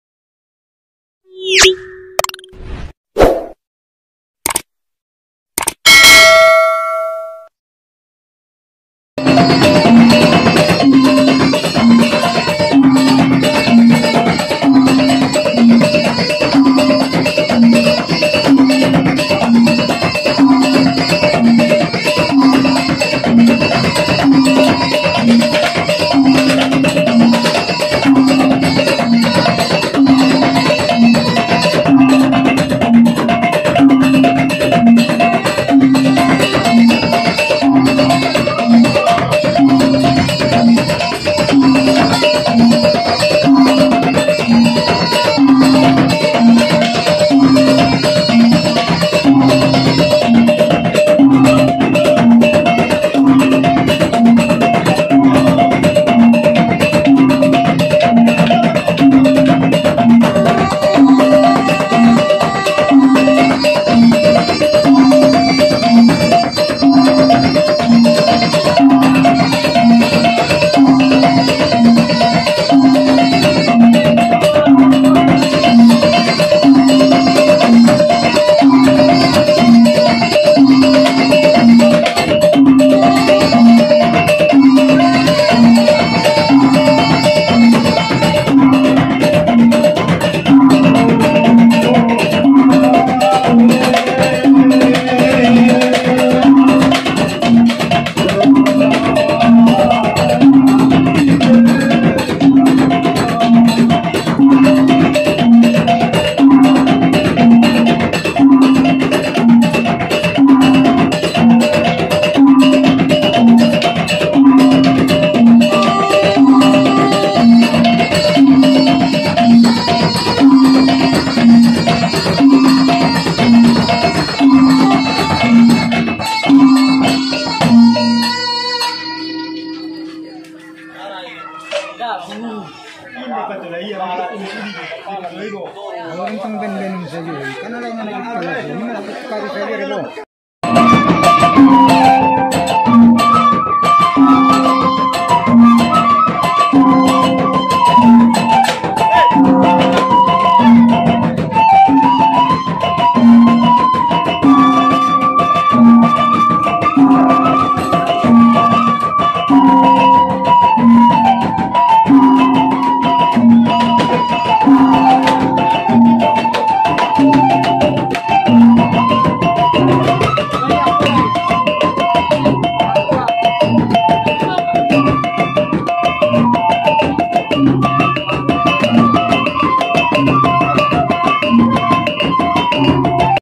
Terlalu Semangat Sampe Kesurupan Gordang Sambilan Barbaran
Gordang Sambilan sangat meriah pemainnya begitu semangat hingga ada yang kesurupan